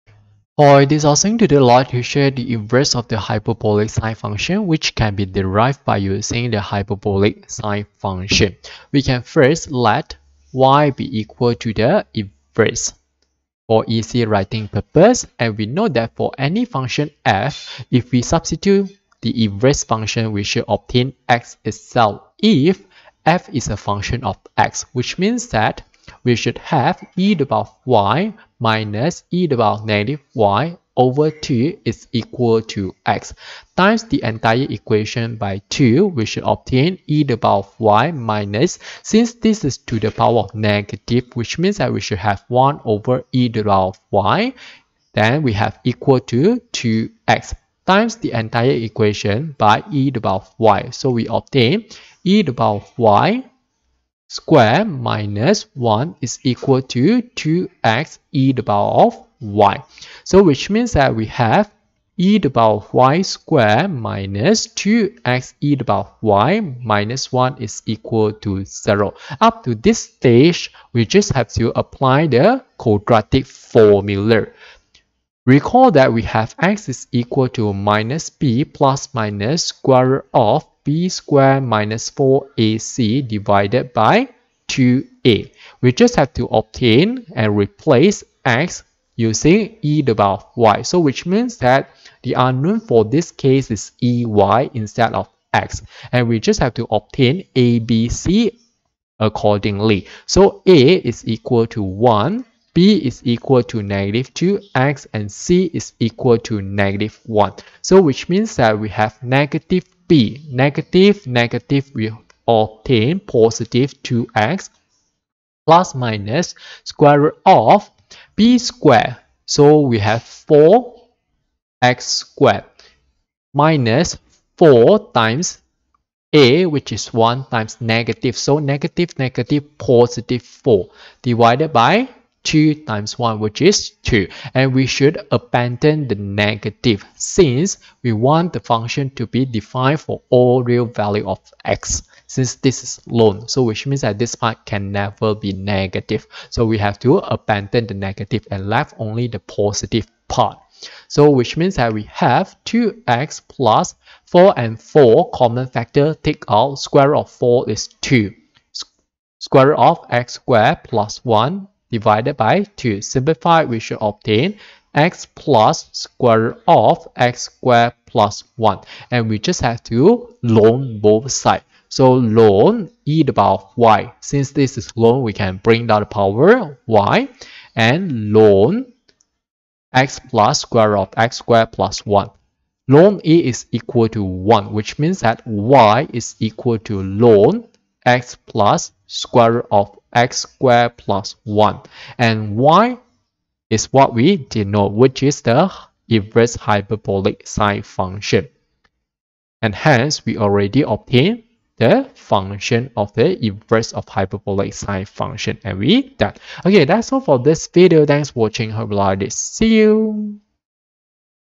Inverse function of sinh x
What is the inverse function of hyperbolic sine function?
Proof of the formula of the inverse hyperbolic sine function.
sinh^-1 (x) = ln(x+sqrt(x^2+1))